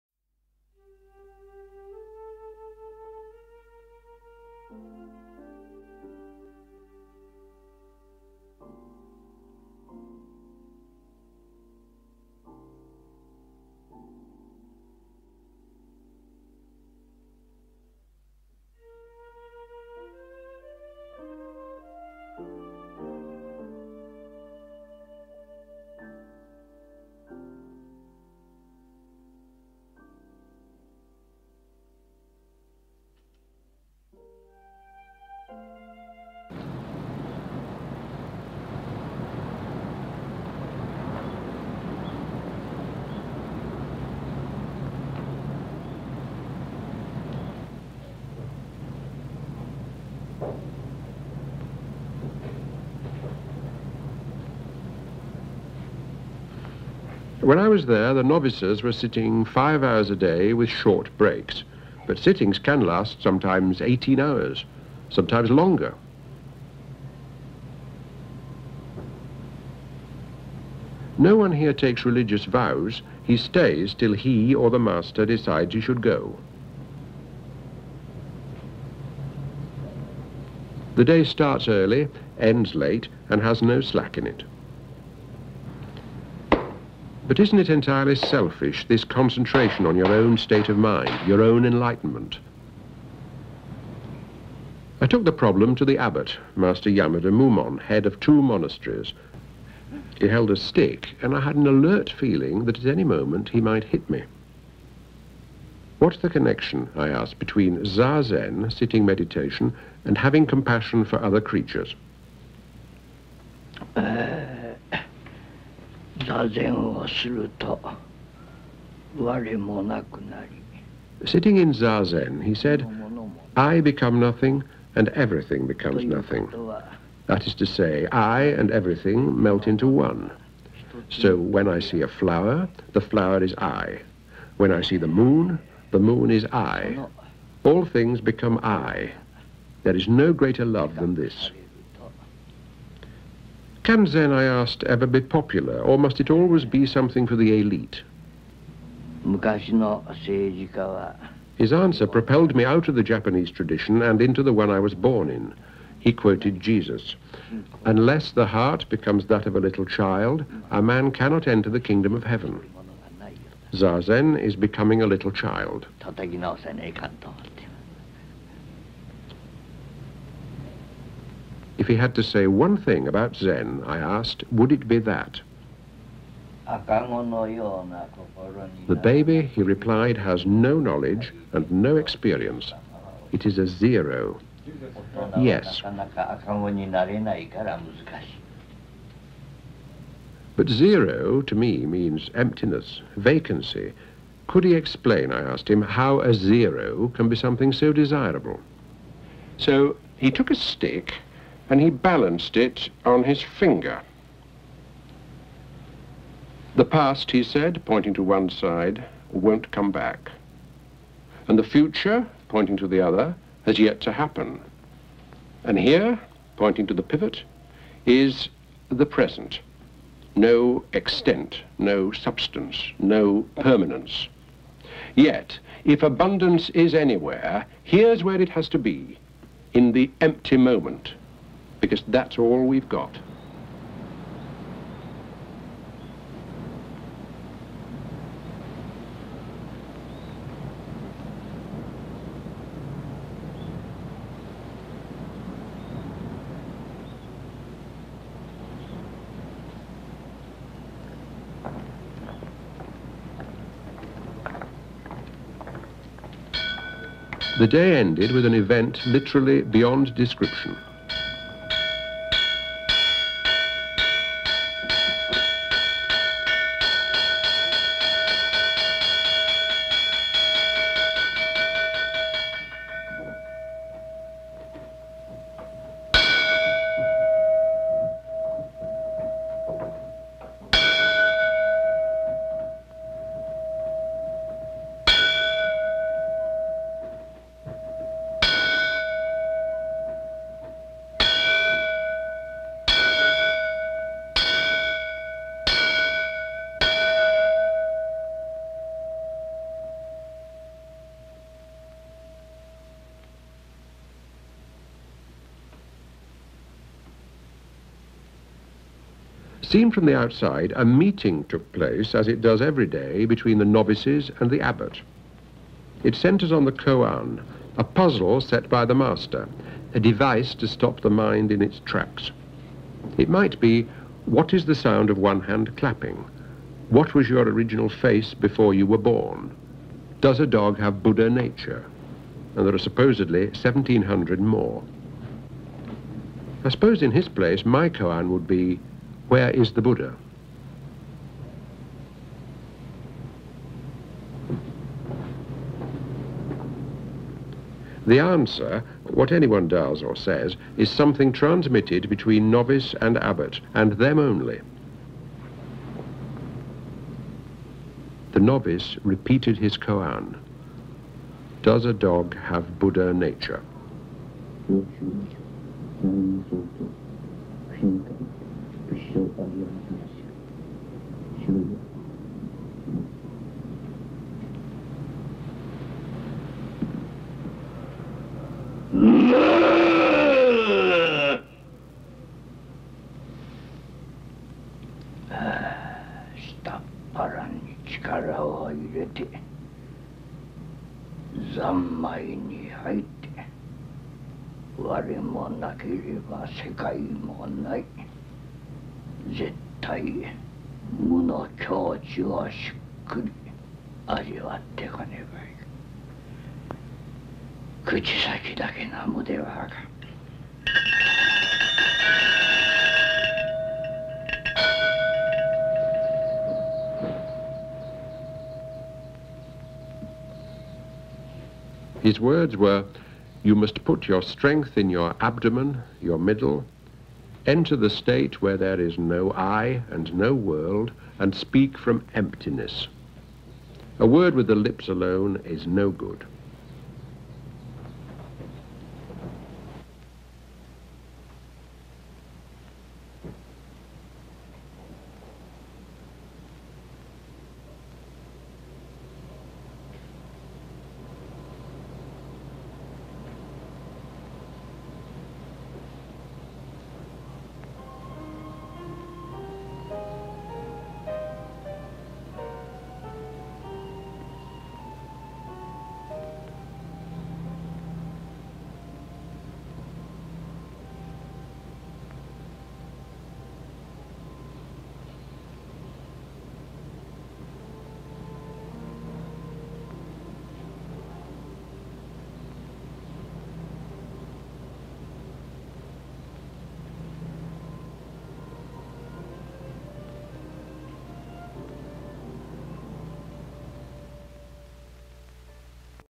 Yamada Mumon Roshi : En zazen je deviens rien et toute chose devient rien
Le grand maitre du Rinzaï  Zen, Yamada Mumon Roshi, filmé en 1977 dans son temple à Kobe : une interview permettant de voir un maitre exceptionnel. Un document rare.